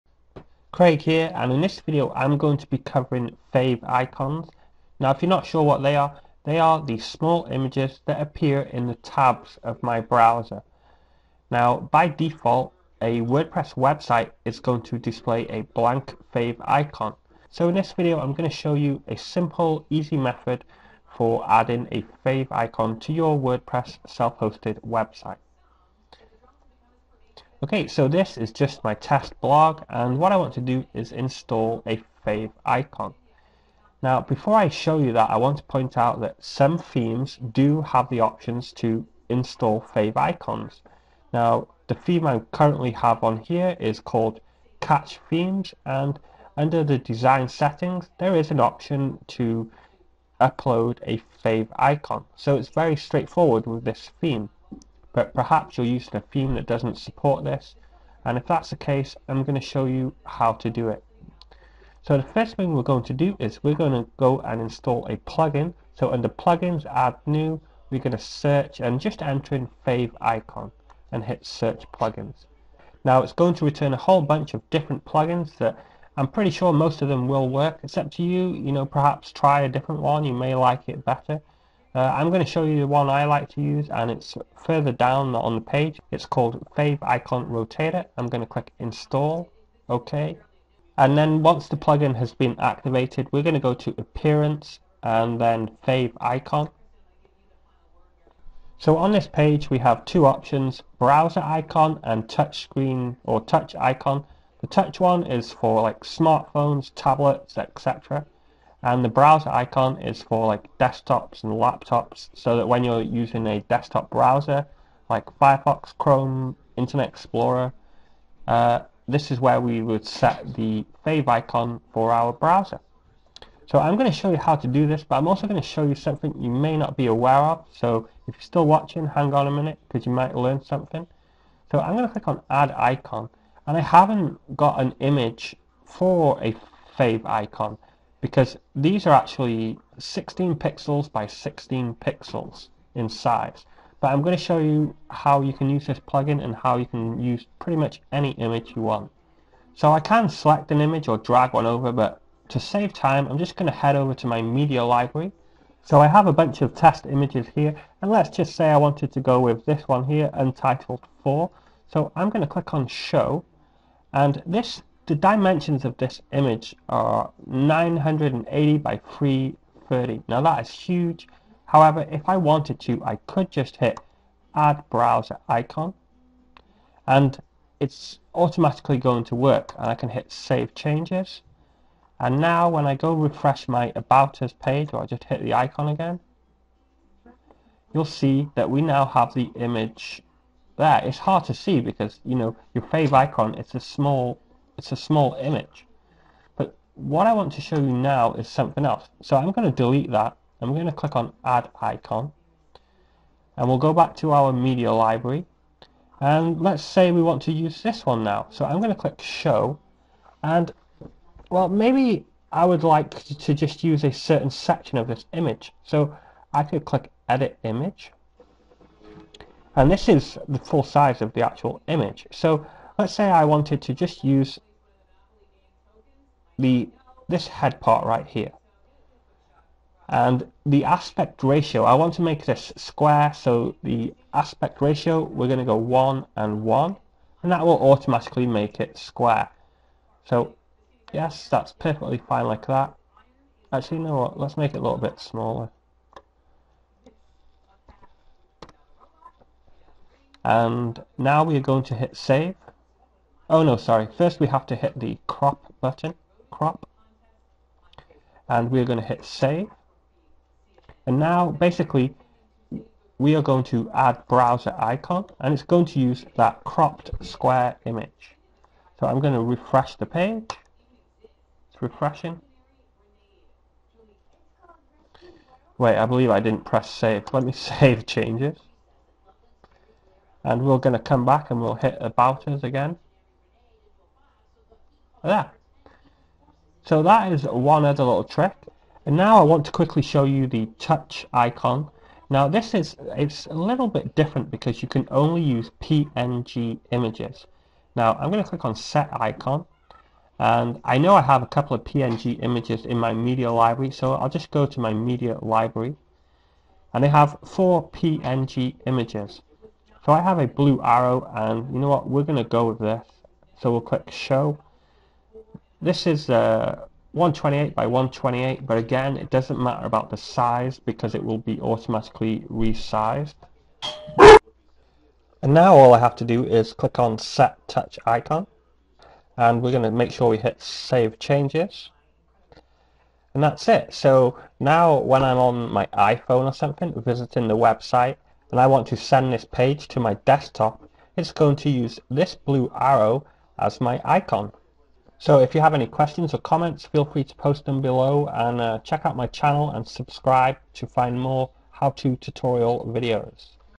How to Add Favicon to Wordpress Site 2013 HD
This video will show you how I personally add a favicon to my sites using a wordpress plugin called "Favicon Rotator".

The reason I like this plugin is because I can use any size image and not have to try create my own 16px X 16px favicon  This plugin also gives you the option to set icons for Touch screen devices like Android, iPhone, iPad, iPod Touch.

If you're wondering why it's called "Favicon Rotator" it's because it also support multiple favicon icons that are randomly displayed.

Before installing any favicon plugin you should check your theme custom option, many wordpress themes have built-in options for adding a favicon.

If you'd like learn how to make a favicon or create your own favicon you should checkout this cool